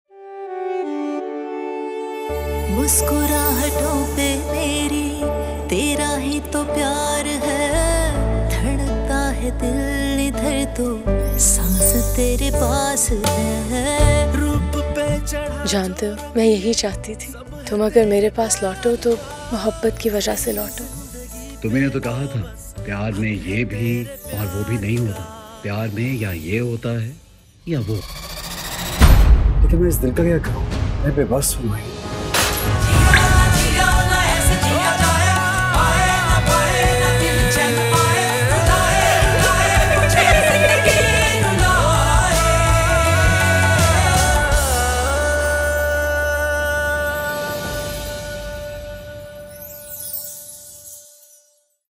Coming Soon | New Drama | Promo | Ahsan Khan | Ushna Shah | Hina Altaf | HAR PAL GEO
Here's another look of our new drama, starring Ahsan Khan alongisde Ushna Shah and Hina Altaf.

More updates to follow soon.